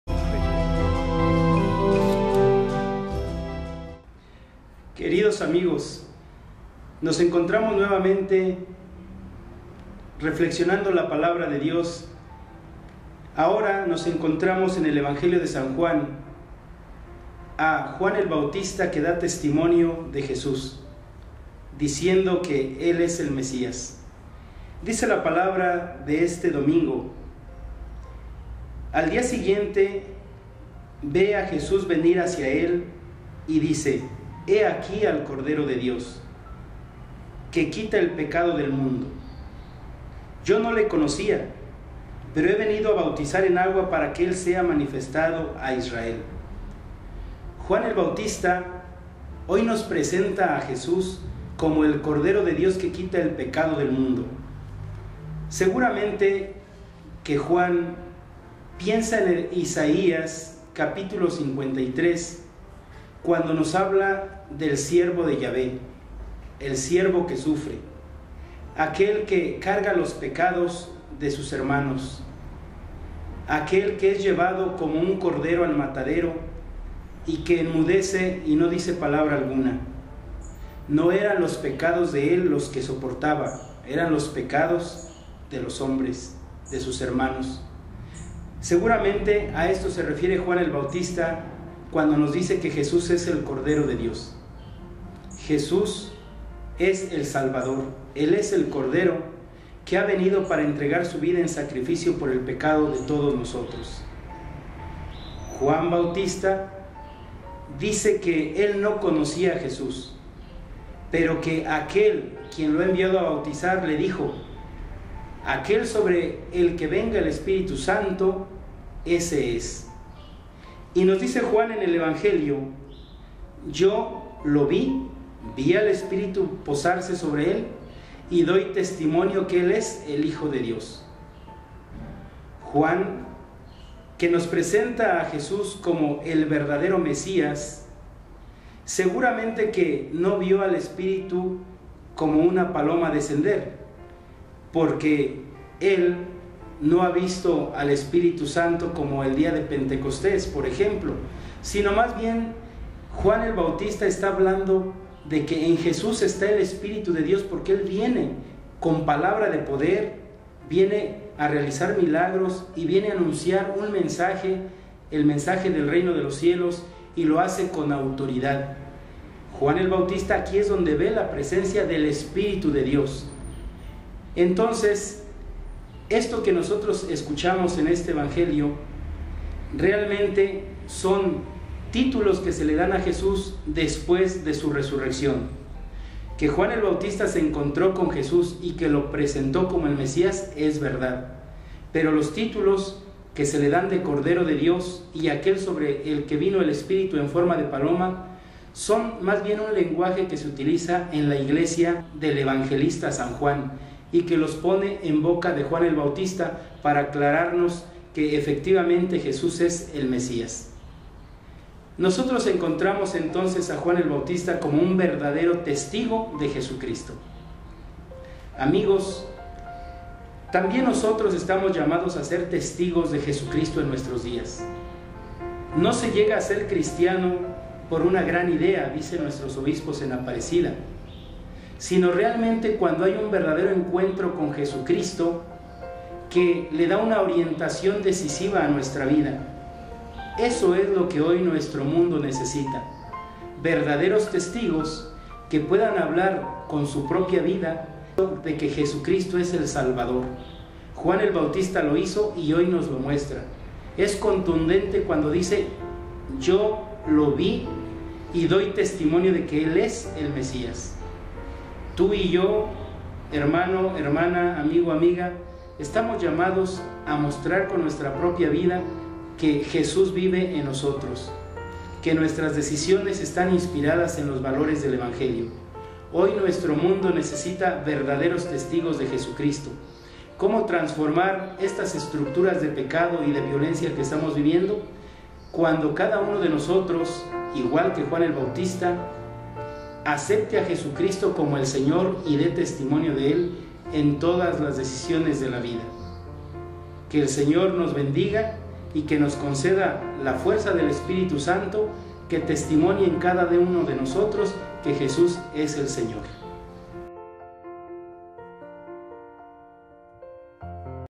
Reflexión sobre el Evangelio del II Domingo del Tiempo Ordinario
Reflexión sobre el Evangelio del II Domingo del Tiempo Ordinario por el Pbro. Lic. José Luis Salinas Ledesma, Presidente de la Comisión Diocesana para la Pastoral Profética de la Diócesis de Querétaro.